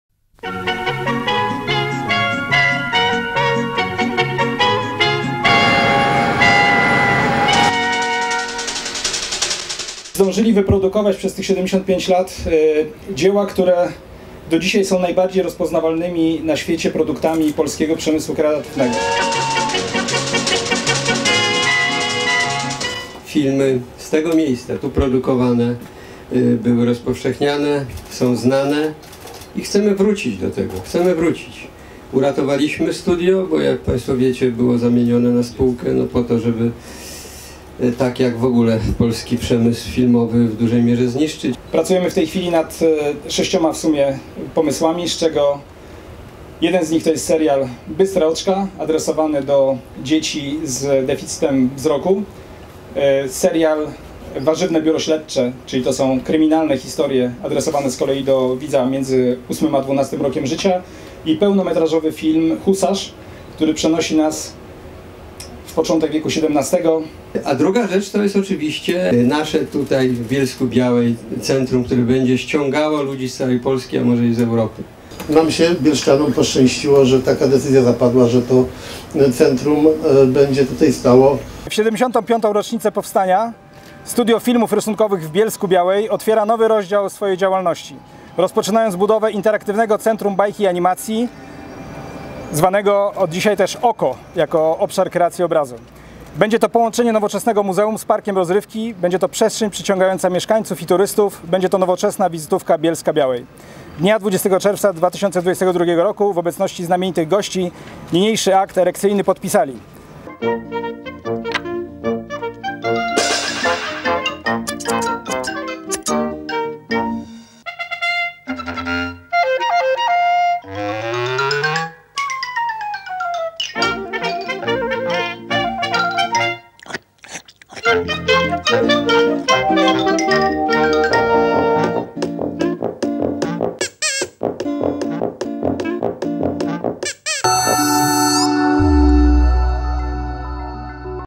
Studio Filmów Rysunkowych i wmurowanie kamienia węgielnego pod nową inwestycję.
Bolek i Lolek, Reksio i Baltazar Gąbka mają już nowych przyjaciół. Animatorzy pracują nad nowymi filmami, a jesienią ma już powstać nowe bajkowe centrum rozrywki.